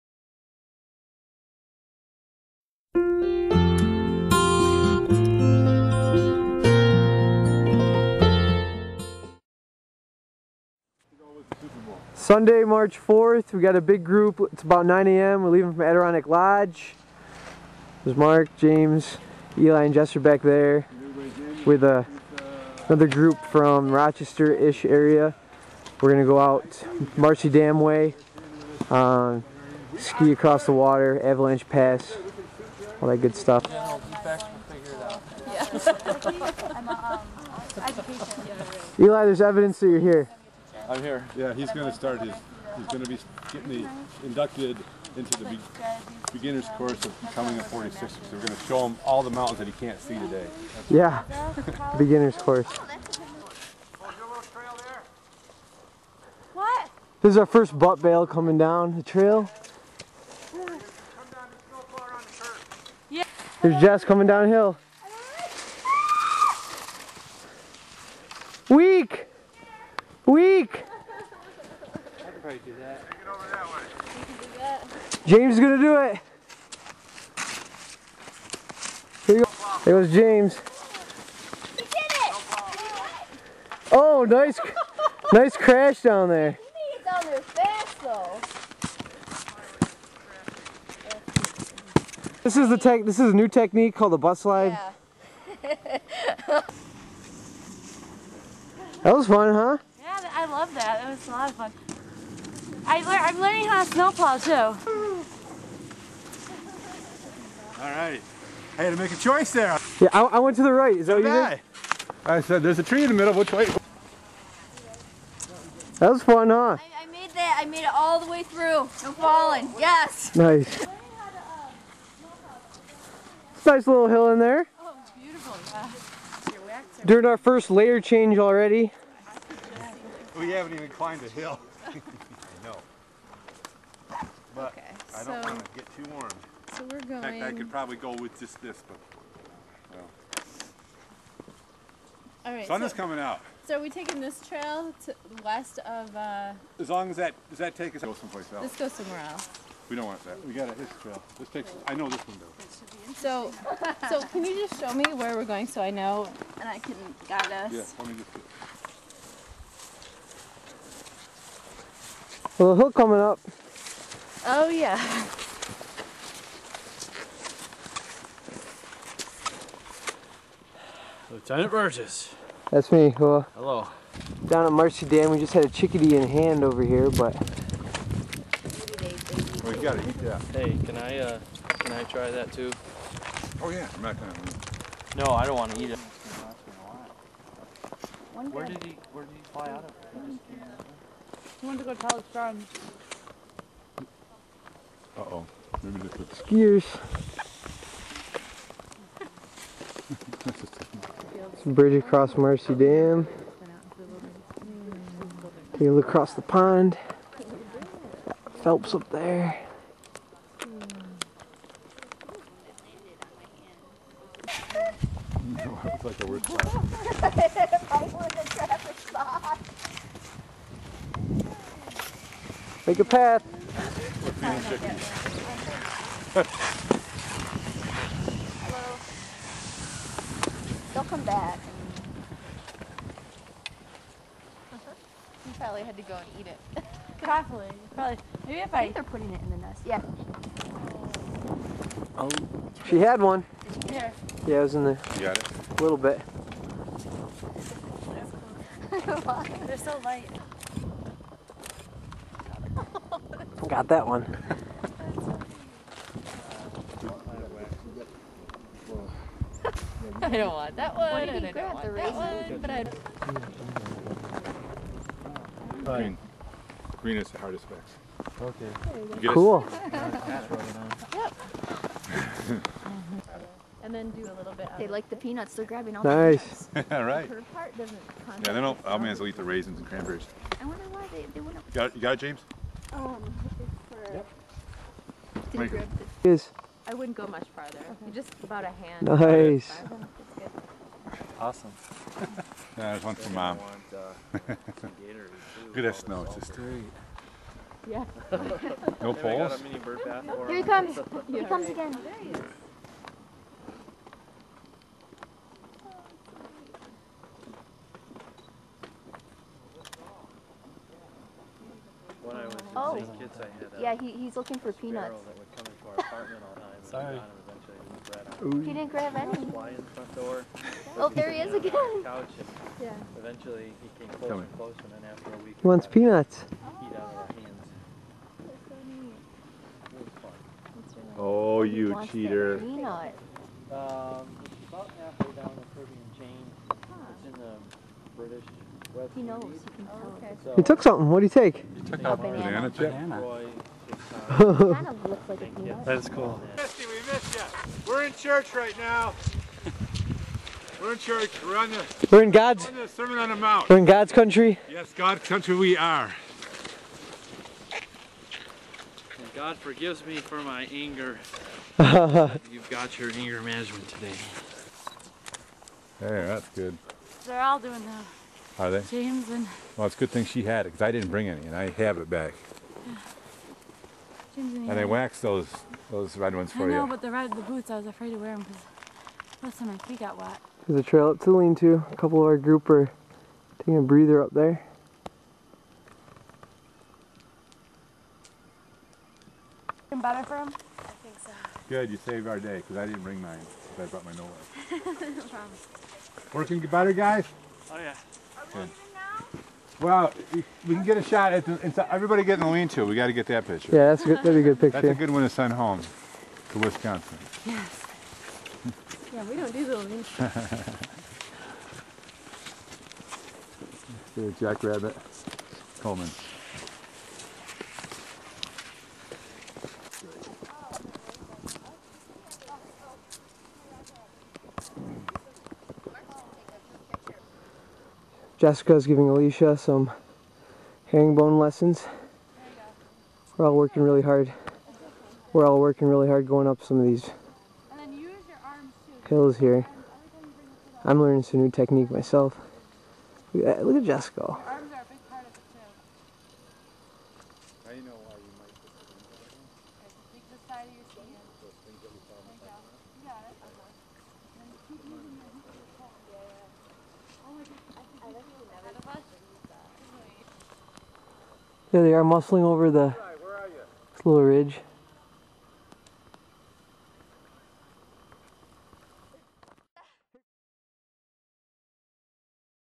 A Trip of a Life Time
A Trip of a Life Time skiing from Adrondack Lodge to Avalanche pass. "This is why we love Winter"